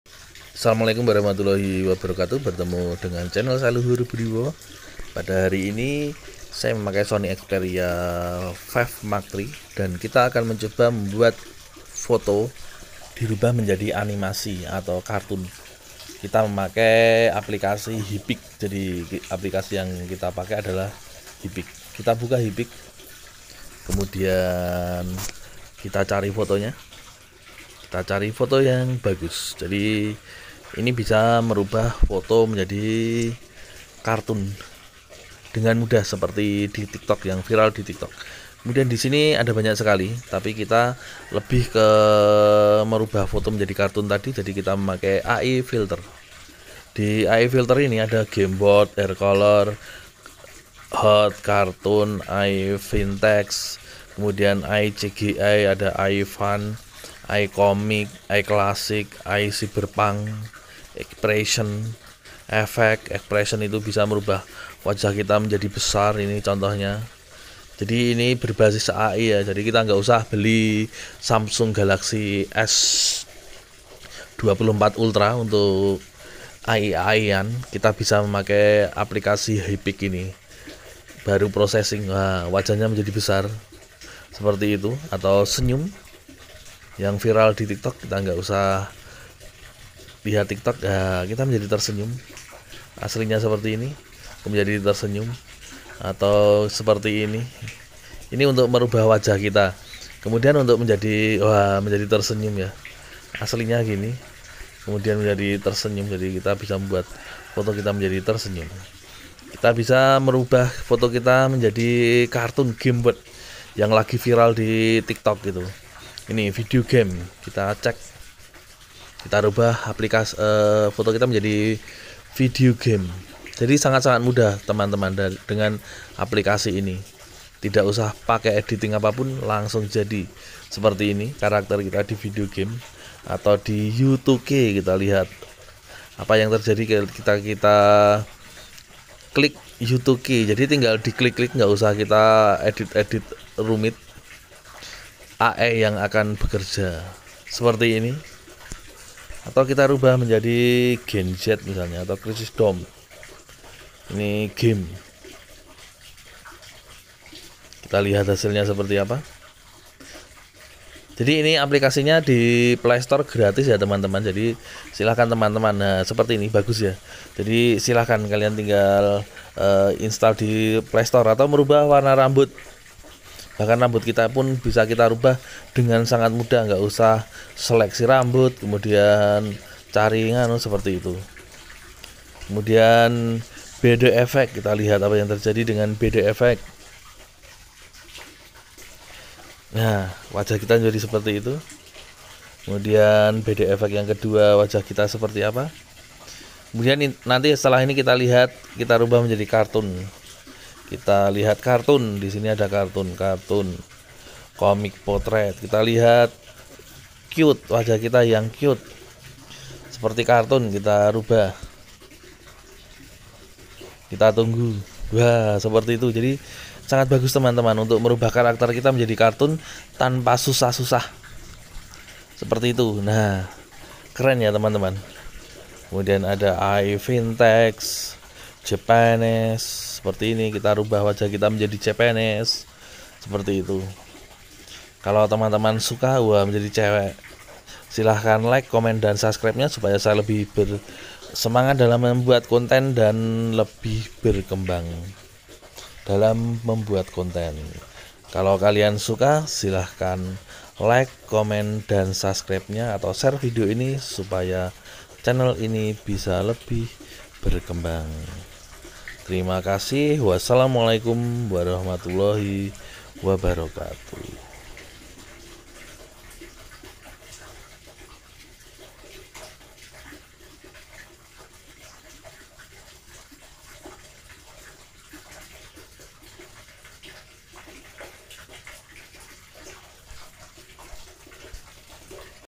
Cara Merubah Foto Kita Menjadi Karakter Kartun atau Video Game yang Viral di Tiktok dengan Mudah
Cara Merubah Foto Kita Menjadi Karakter Kartun atau Video Game yang Viral di Tiktok dengan Mudah. Ai sekarang ada dimana-mana.
Sangat susah menentukan mana foto natural, foto asli, atau foto rekayasa.
Semua karena teknologi AI.
Berikut cara yang sedang viral di tiktok yaitu merubah foto menjadi animasi atau karakter kartun.

Kalau kalian suka silahkan like, komen, dan subscribenya supaya chanel ini semakin berkembang.
Terima kasih